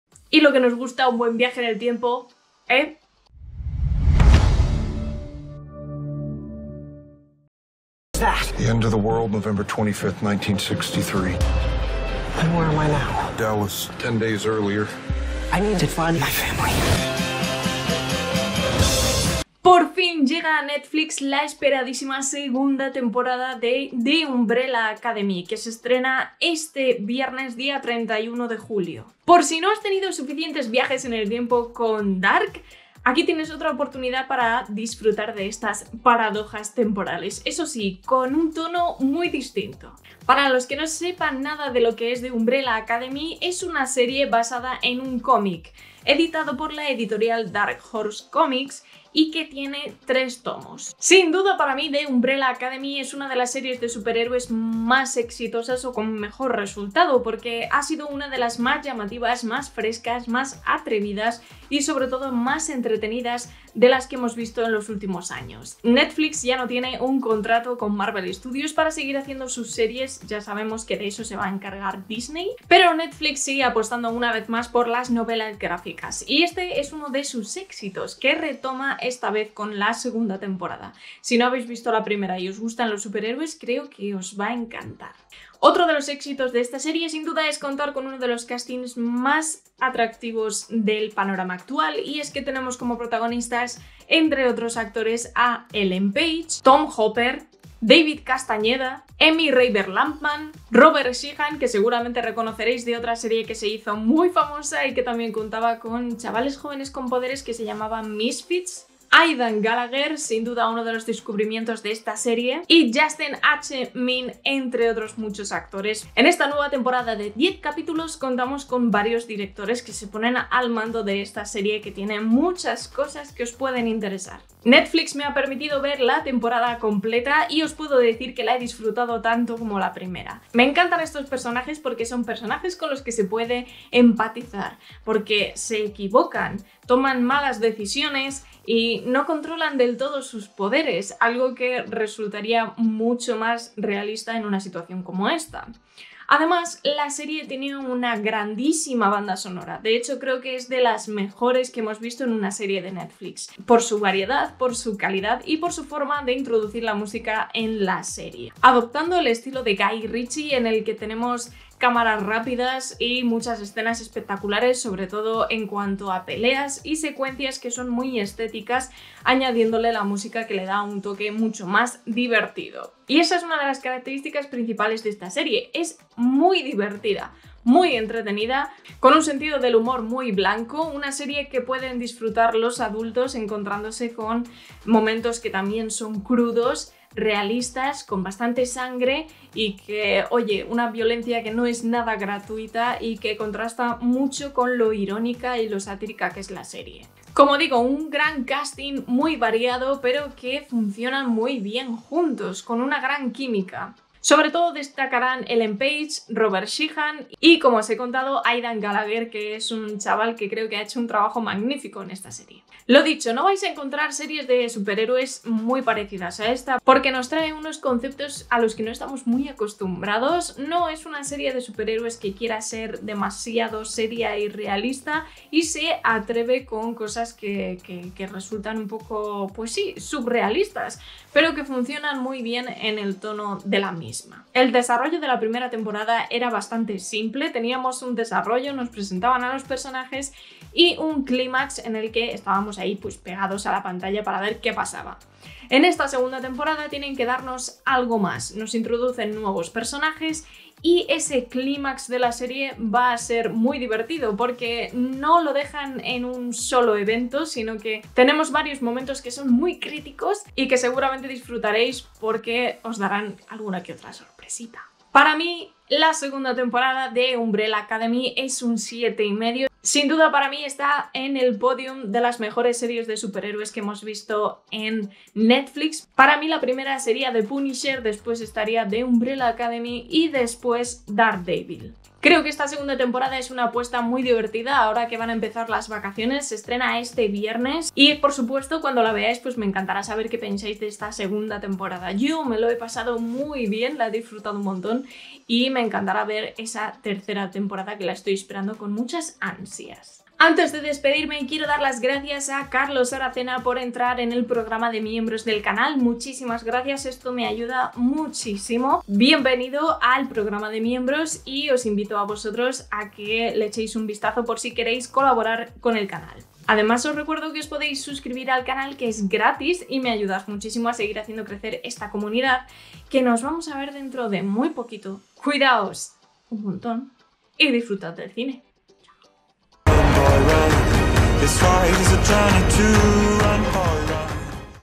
Crítica - 'The Umbrella Academy' / Segunda temporada /Netflix/ (Sin spoilers)
Mi opinión / crítica / reseña de la nueva segunda temporada de la serie 'The Umbrella Academy' en Netflix. Protagonizada por Ellen Page, Robert Sheehan, Tom Hopper, Aidan Gallager, David Castañeda, Emmy Raver Lampman y Justin H. Min.